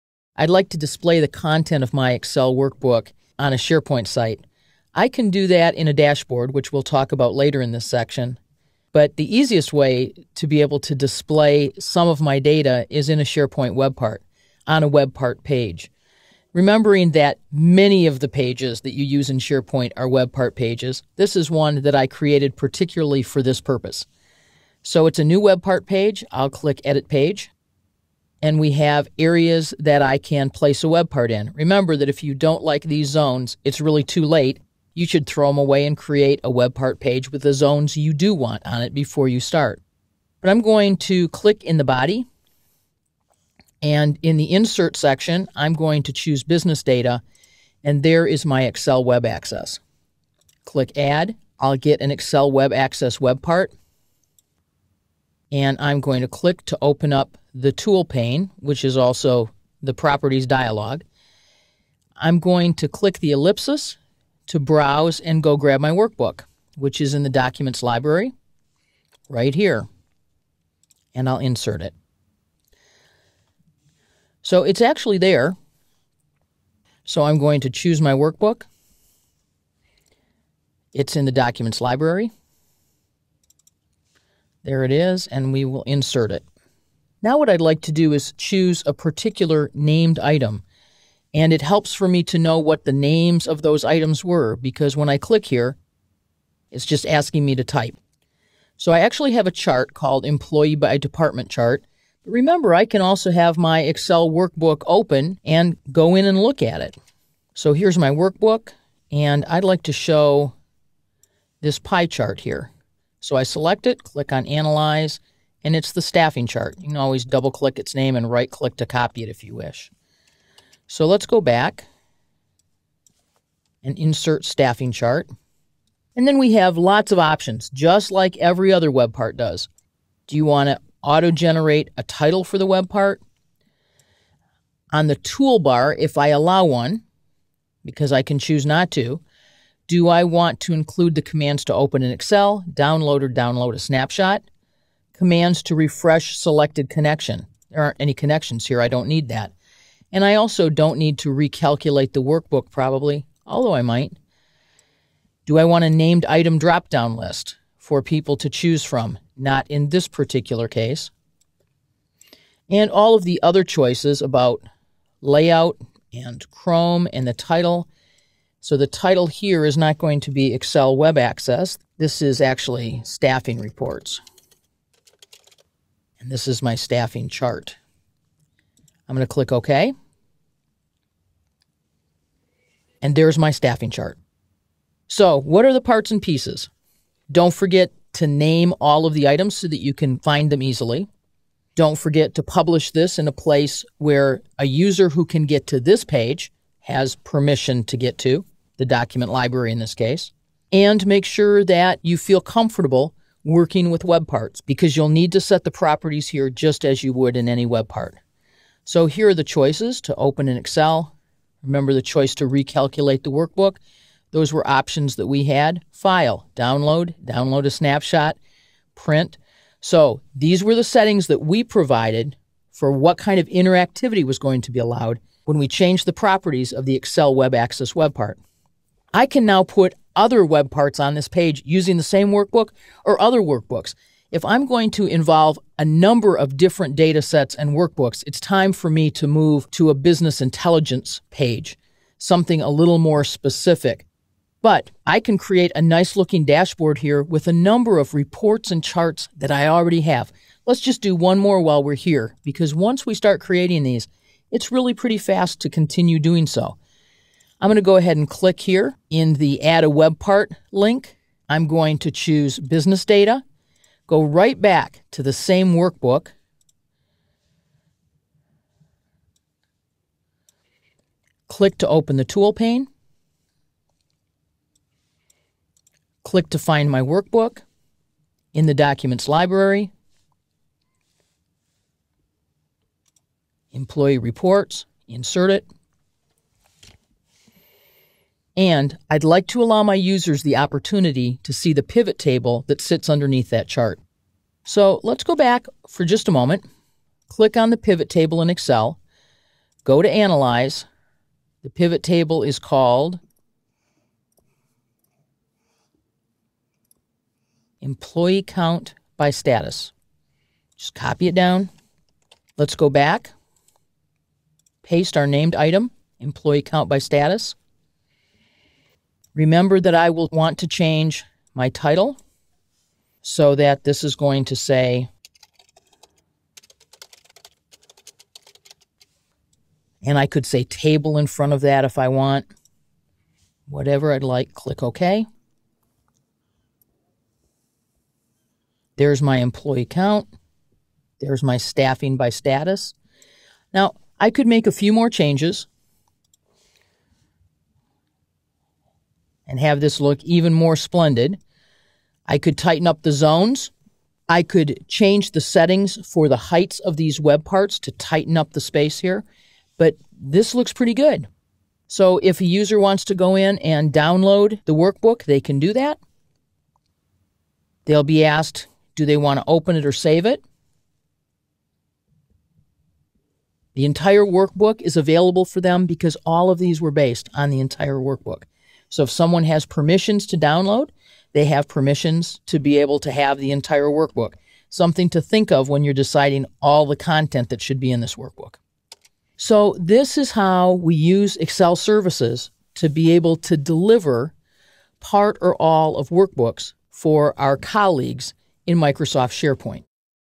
SharePoint Tutorial - Sharing a workbook using an Excel Web Access Web Part
LinkedIn Learning is the next generation of Lynda.com. Grow your skills by exploring more SharePoint courses today